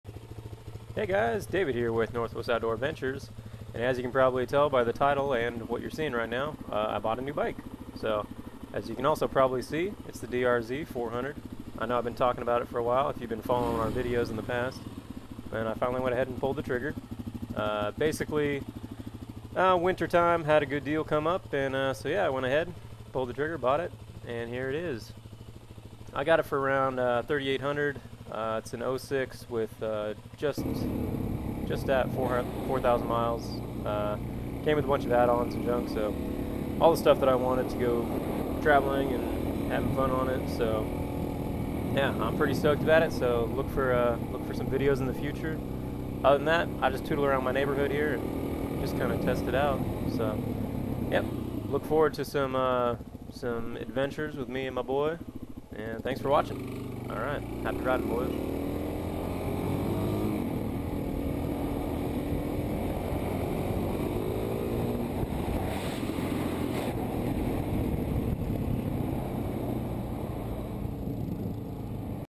New Bike: Suzuki DRZ400s
I bought a new bike, check it out dudes.